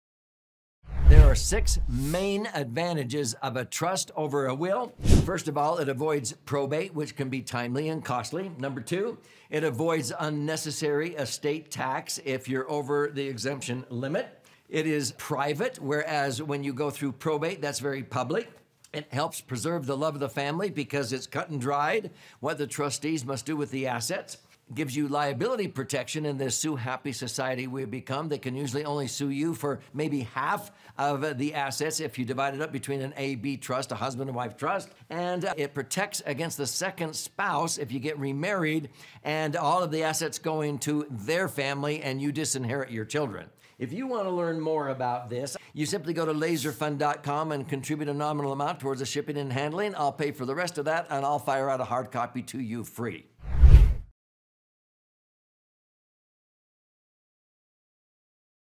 Trusts Over Wills
People often ask - what's better for the future of the family - setting up a trust or creating a will?

My answer? A trust, hands down. Here's why:

🔒 You Can Avoid Probate: Trusts provide a smoother transition of assets without the need for the often cumbersome and costly probate process.

📉 You Can Save on Unnecessary Estate Taxes: Strategically managing assets through a trust can help eliminate unnecessary estate taxes if you're over the exemption limit.

🕵️ You Can Enjoy Greater Privacy: Unlike probate proceedings, trusts operate discreetly, preserving your family's privacy.

👨‍👩‍👧‍👦 Your Family Can Get Avoid Conflict: Clear directives within a trust can help maintain family unity by outlining precisely how trustees should handle assets.

🛡️ You Can Have Greater Liability Protection: In a litigious society, trusts can shield a significant portion of assets, making it more challenging for potential litigants to access your entire estate.

🛡️ Safeguarding Family Interests: Trusts act as a protective shield, ensuring that assets are distributed according to your wishes and preventing unintended consequences like disinheriting children in case of remarriage.

Ready to delve deeper into the world of trusts? DM me and my team will get back to you.